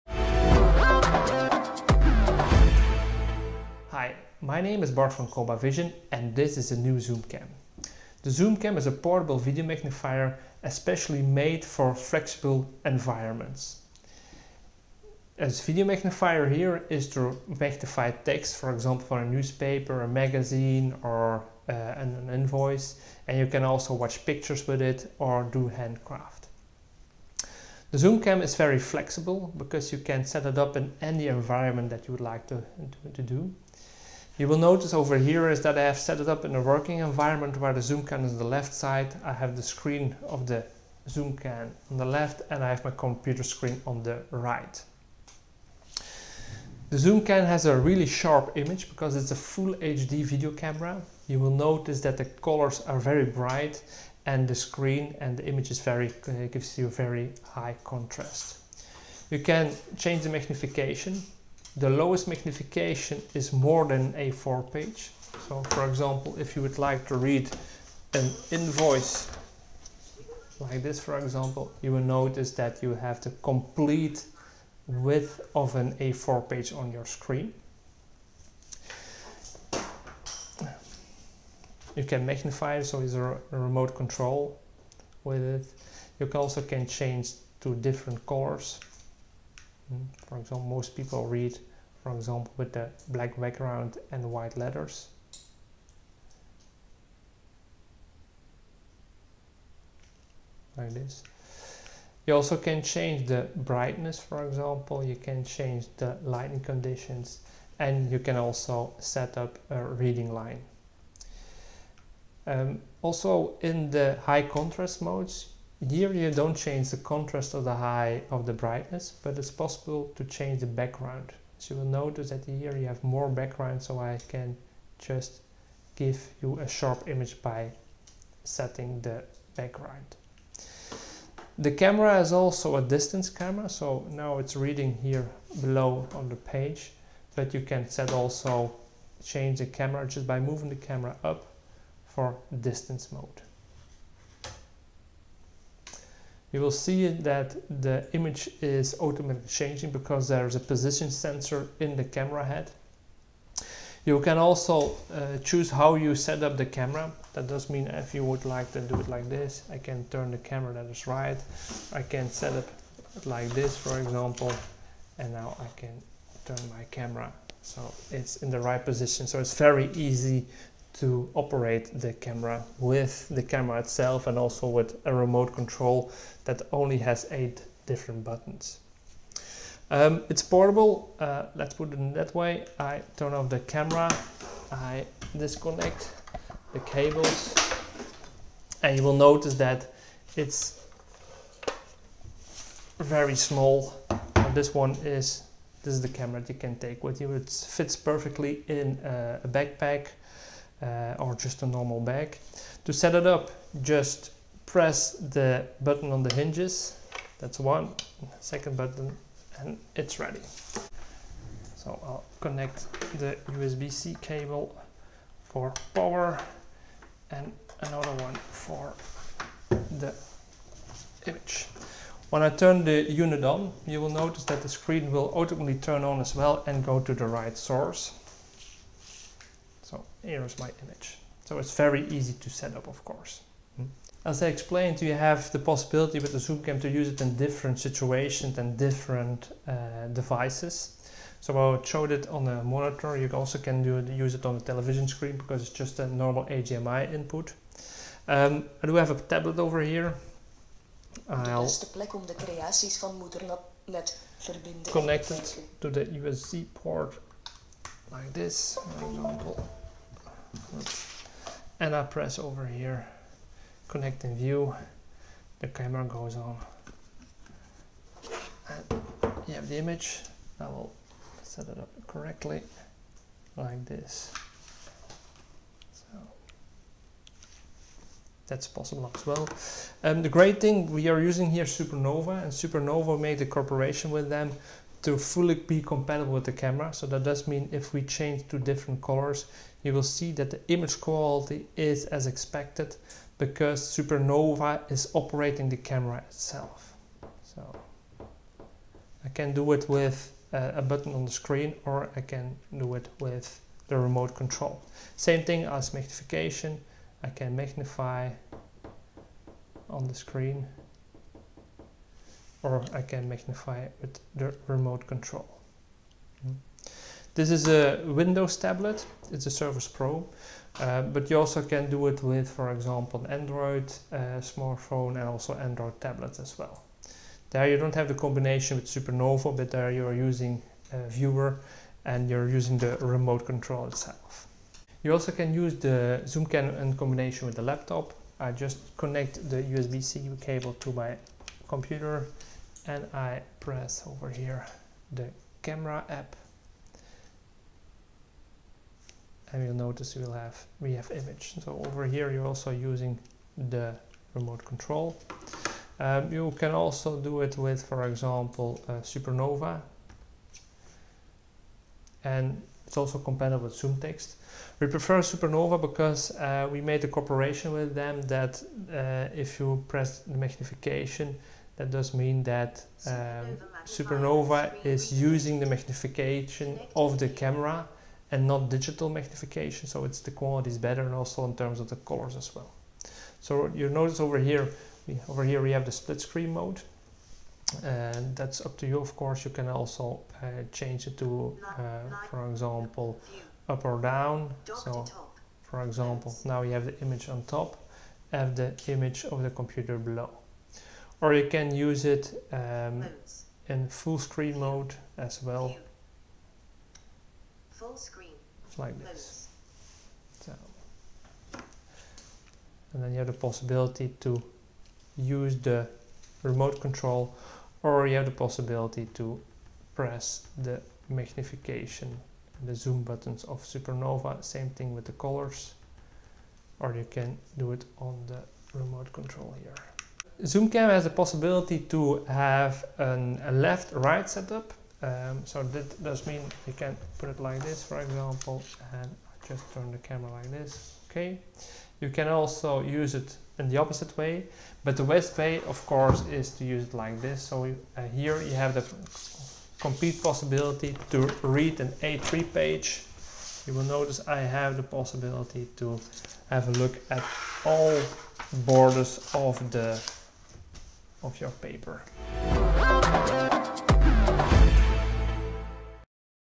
ZoomCam Portable Camera Solution
ZoomCam is a professional transportable video magnifier which can be used anywhere in combination with a computer, screen or tablet. Ideal for magnifing text in newspapers, books and invoices or for looking at pictures and objects. A discrete solution which looks like a desk lamp. ZoomCam sets up quickly, is compact and easy to use. The sharp and stable Full HD image provides an optimal reading experience. In addition to its high display quality, Zoomcam has a 23cm extra wide viewing angle allowing one to read across a full page.